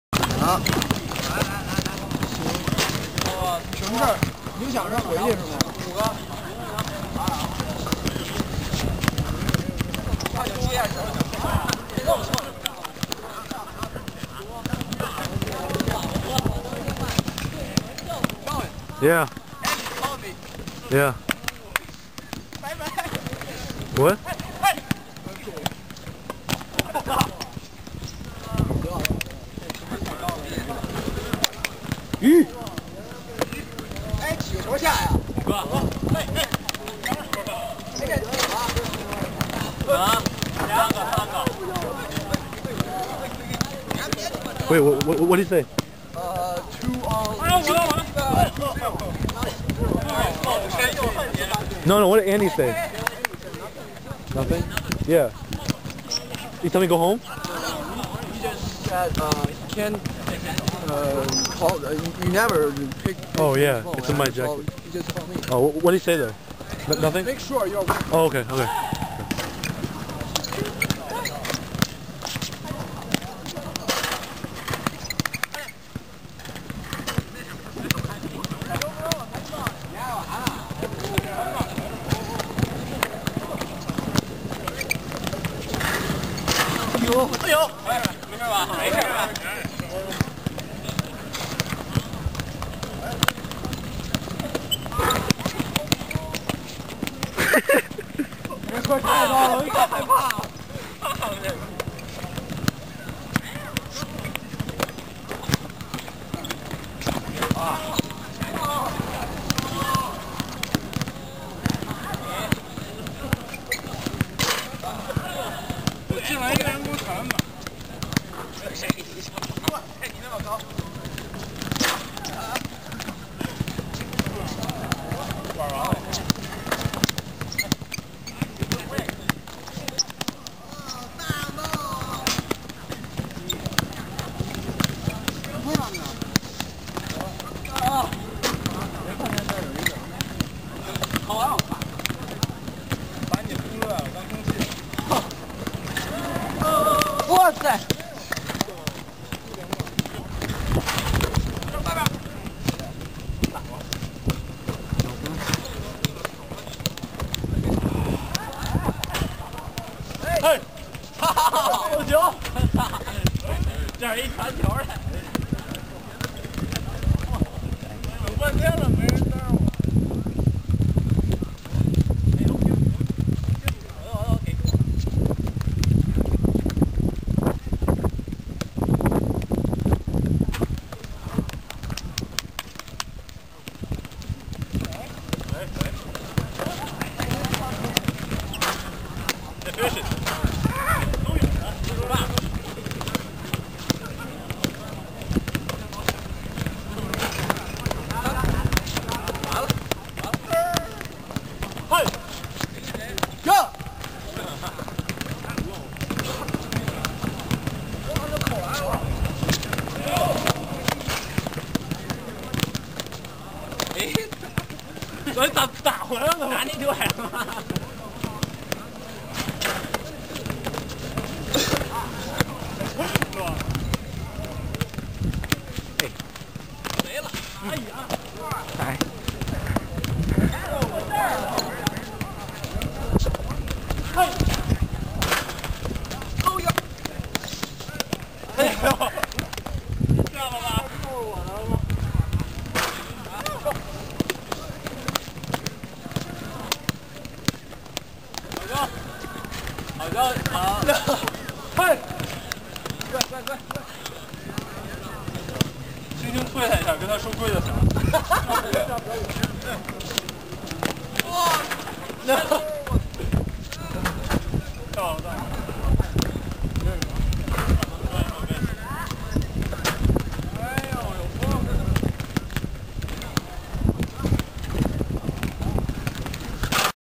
bball at Jingshan
played alot of bball these past 2 days. crazy kid in this video is like 6ft 8...
CRAZY. cant stop him. ok... more videos to come later.